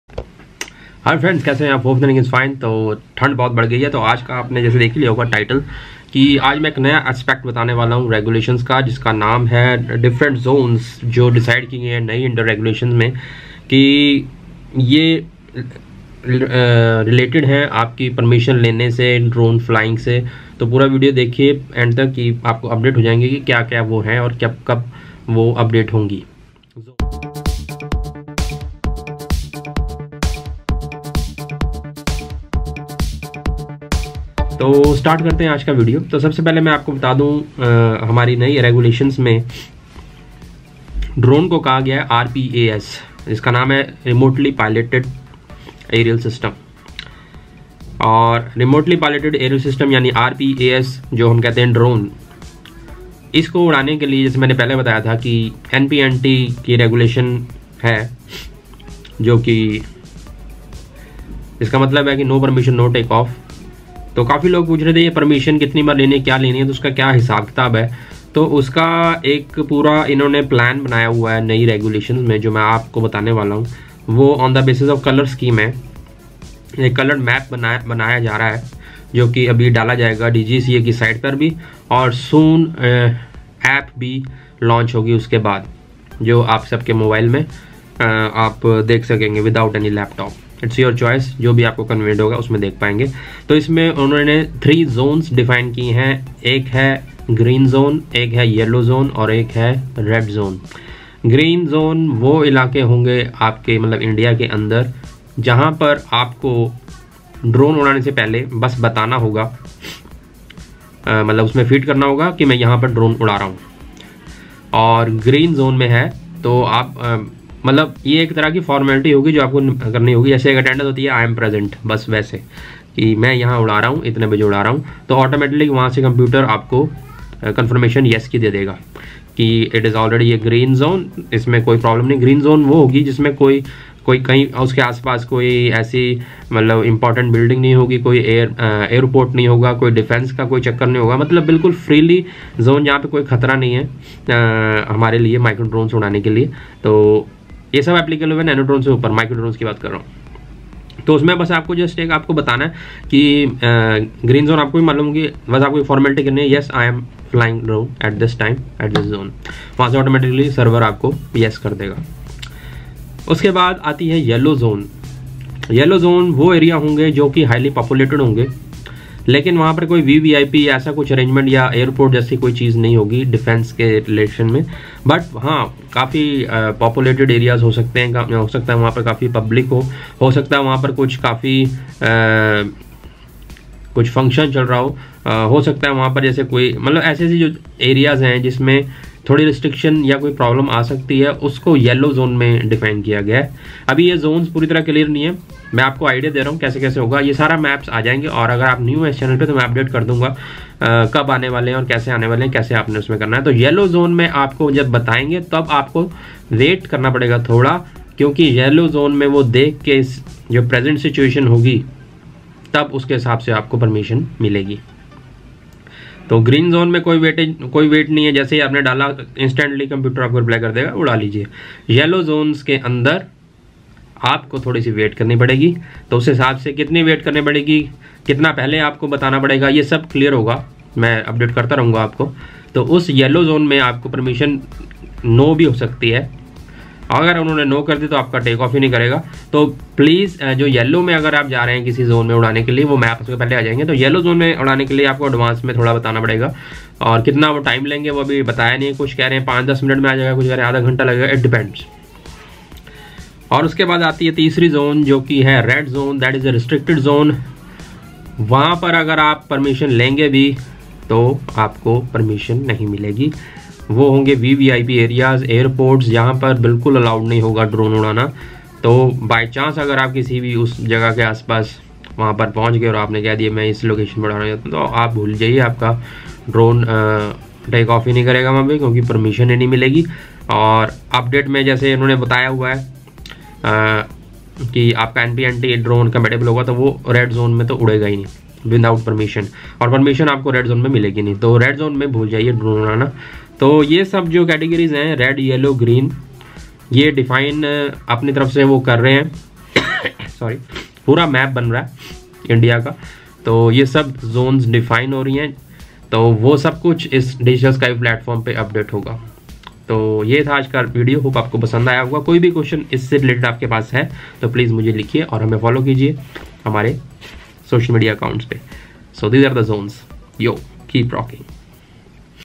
What are colored flying zones in Indian drone regulations
This video is about about colored flying zones in final Indian regulations.

Microphones:
zoom h2n